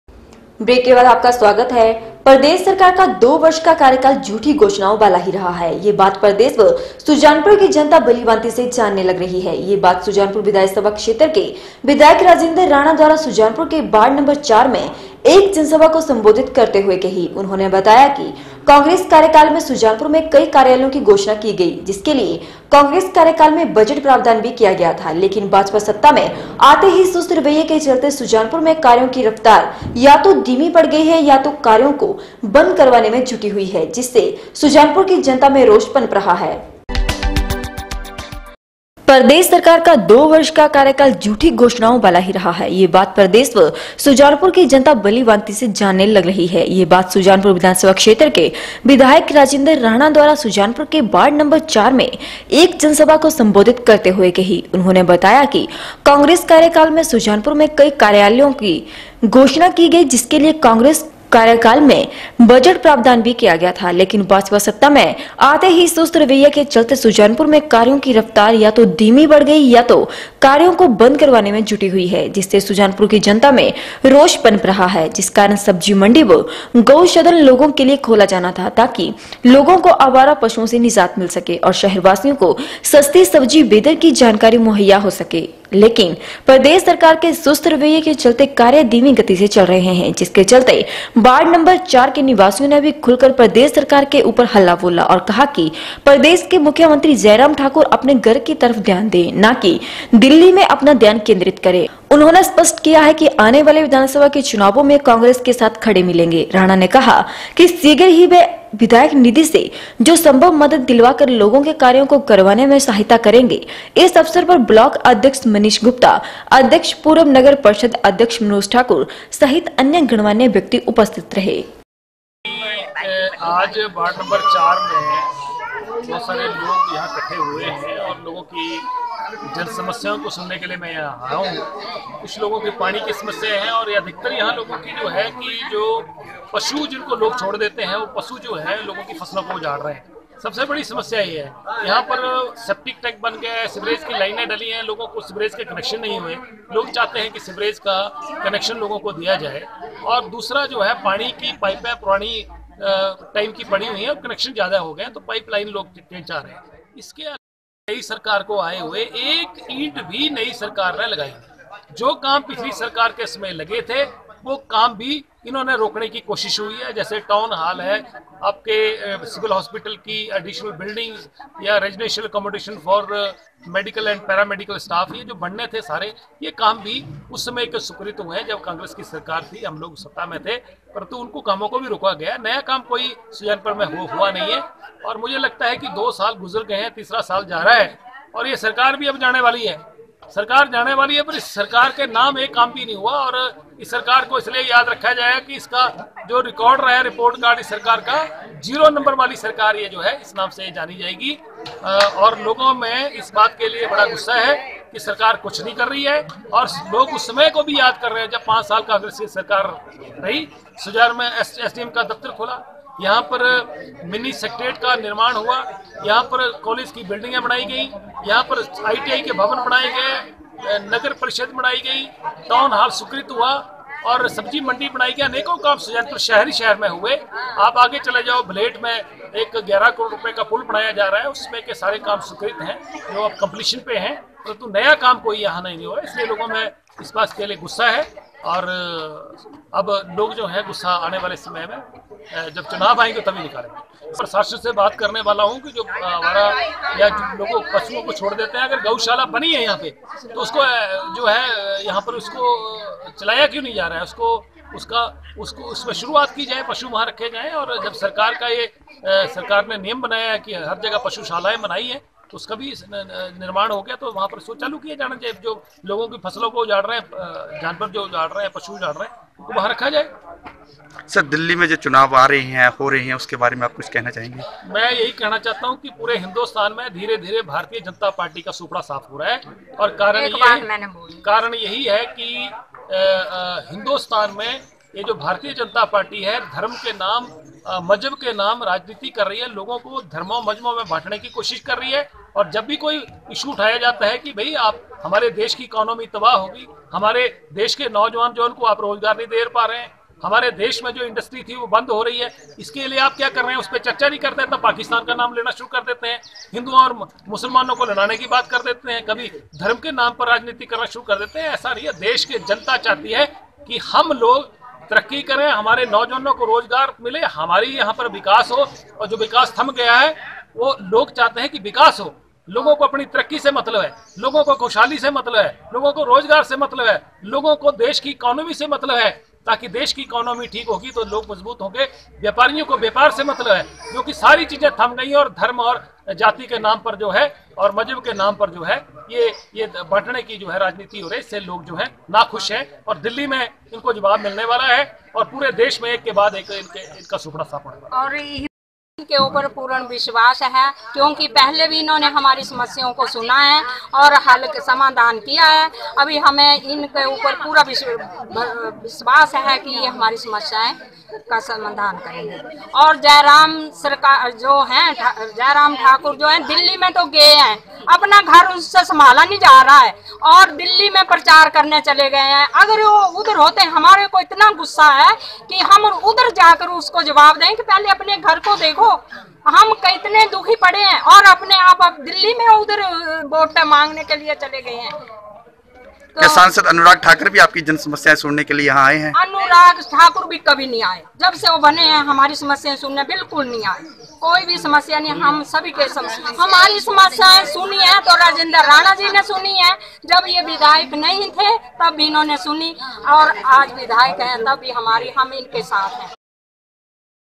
10 FEB N 4 प्रदेश सरकार का 2 वर्ष का कार्यकाल झूठी घोषणाओं वाला ही रहा
प्रदेश सरकार का 2 वर्ष का कार्यकाल झूठी घोषणाओं वाला ही रहा है यह बात  प्रदेश व सुजानपुर की जनता भलीभांति से जानने लग रही है  यह बात सुजानपुर विधानसभा क्षेत्र के विधायक राजेंद्र राणा द्वारा ,सुजानपुर के वार्ड नंबर 4 में एक जनसभा को संबोधित करते हुए कही ,उन्होंने बताया कि कांग्रेश कार्यकाल में सुजानपुर में कई कार्यालयों की घोषणा की गई जिसके लिए कांग्रेश कार्यकाल में बजट प्रावधान भी किया गया था लेकिन भाजपा सत्ता में आते ही सुस्त रवैया के चलते सुजानपुर में कार्यों की रफ्तार या तो धीमी पड़ गई है या तो  कार्यों को बंद करवाने में जुटी हुई हैं  जिससे सुजानपुर की जनता में रोष पनप रहा है